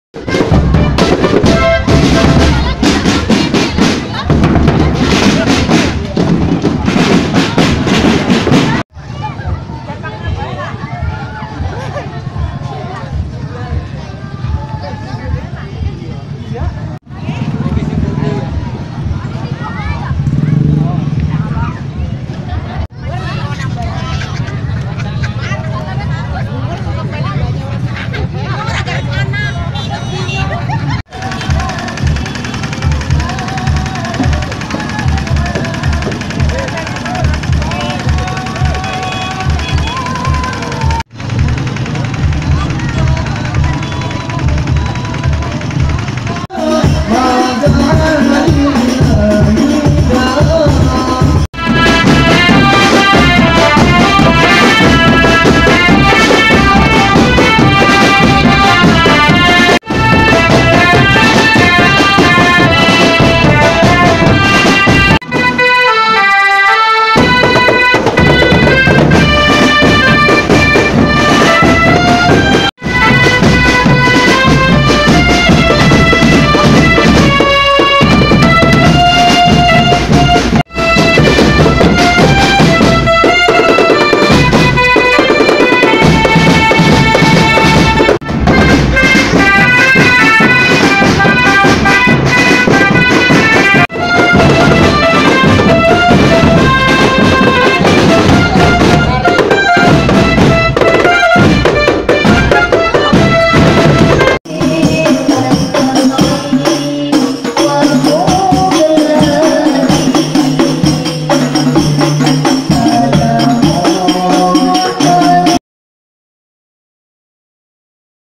Parade Drum Band di Madin Al-Istimror, desa Muktisari (10 Mei 2015)
Dalam rangka Isro' Mi'roj Nabi Muhammad SAW, dan Khotmil Quran Madrasah Diniyah Al-Istimror, Desa Muktisari, Kecamatan Gandrungmangu, Cilacap. Tema yang diusung adalah "Tumbuhkan Gerakan Cinta dan Sayang Sesama"